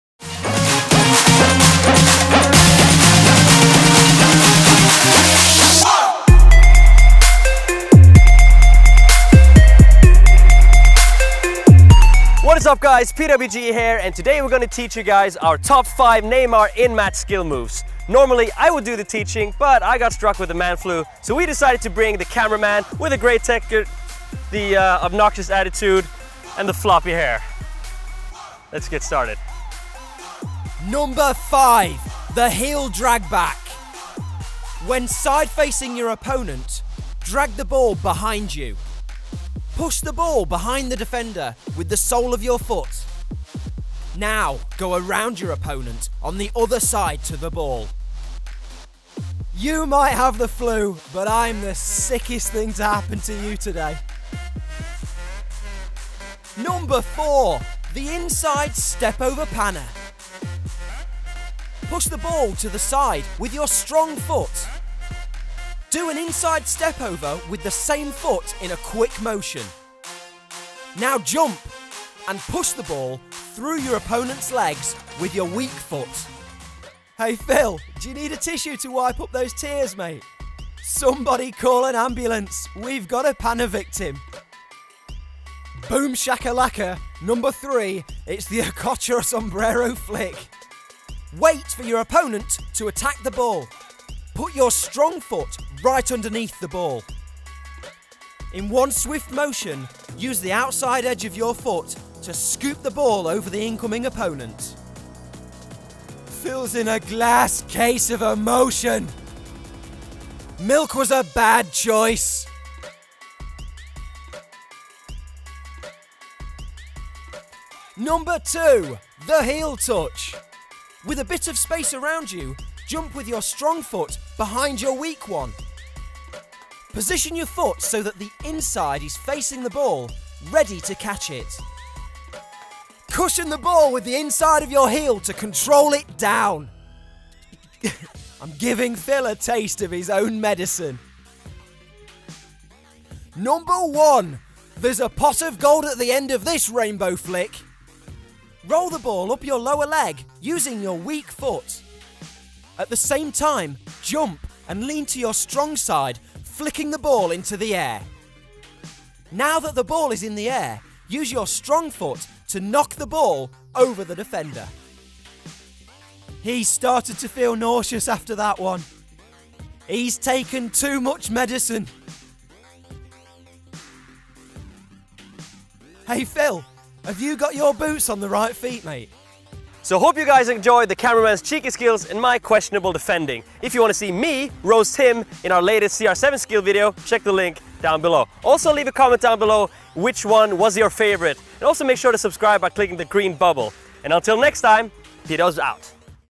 TOP 5 Neymar football skills
Neymar football skills you need to learn - we've chosen the five best and most iconic Neymar football skills, feints and tricks that you should learn. Neymar is one of the best players on the planet at dribbling and dazzling his opponent with his amazing football skills, and if you want to play as Neymar and do some of the most popular Neymar football skills, this is the video to watch. In today's video, PWG looks into the five best football skills done by Neymar on the pitch. Wearing the Nike Mercurial Vapor 11 football boots, the young Brazilian forward has been dropping defenders since he first burst onto the scene for Santos FC. Moving to FC Barcelona and since Paris SG, Neymar has made a name for himself as one of the best football players in the world but also one of the most skilled dribblers in football - and today, you can learn some of those Neymar football skills for yourself.